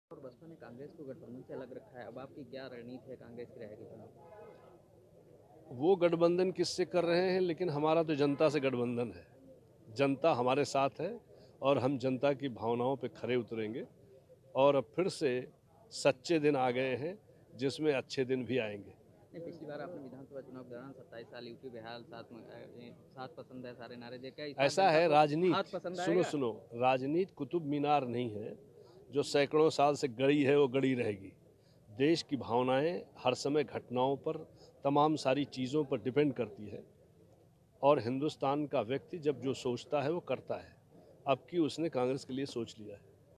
कांग्रेस का गठबंधन जनता से है, वह उसी को जीत दिलाएगी - संजय सिंह
लखनऊ : कांग्रेस कमेटी मुख्यालय पहुंचे कांग्रेस के वरिष्ठ नेता संजय सिंह ने ईटीवी से एक्सक्लूसिव बातचीत में कहा कि सपा और बसपा के गठबंधन में कांग्रेस शामिल नहीं है क्योंकि कांग्रेस का जनता से गठबंधन हो चुका है. जनता कांग्रेस को जीत दिलाएगी.